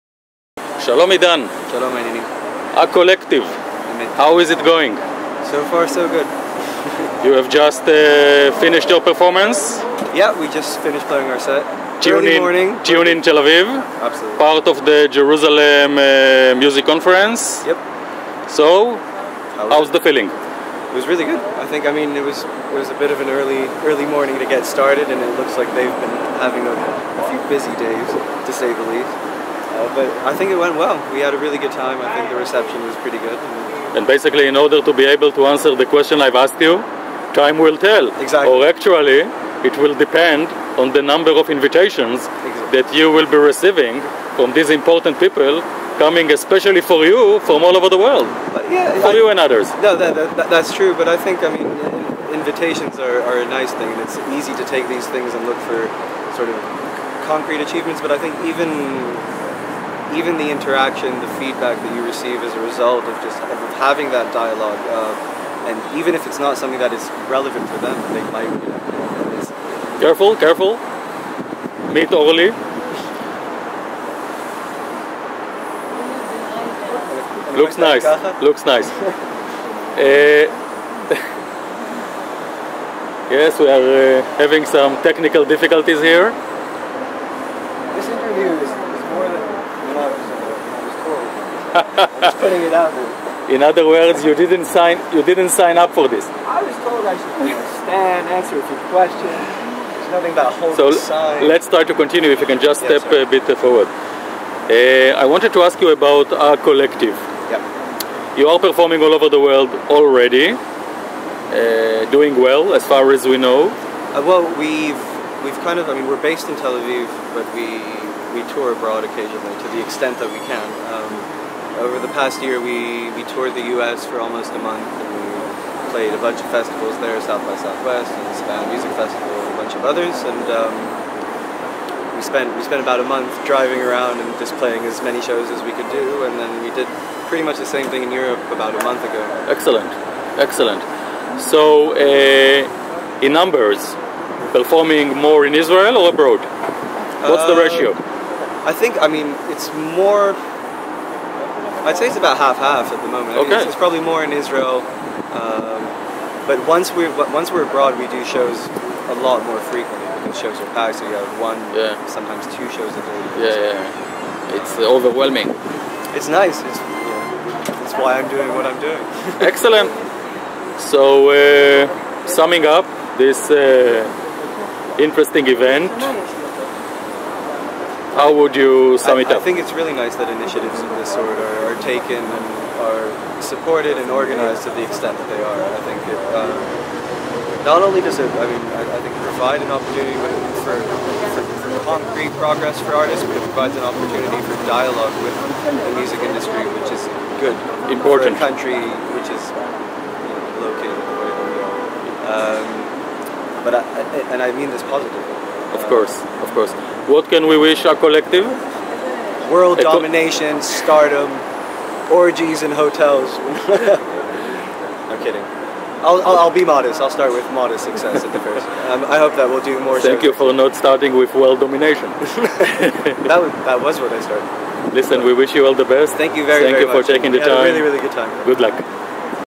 CultureBuzz Converses with ACOLLECTIVE - Tune in Tel Aviv & the World!
"ACOLLECTIVE" is definitely a band to watch (& listen to), so when Music promoters from all over the World gathered @ for the Tel Aviv leg of the historic Jerusalem Music Conference, the band was there, amongst some of the most talented Israeli Musicians, and with full force!

And we had a chance to converse with ACOLLECTIVE's Idan Rabinovici as well (& others too).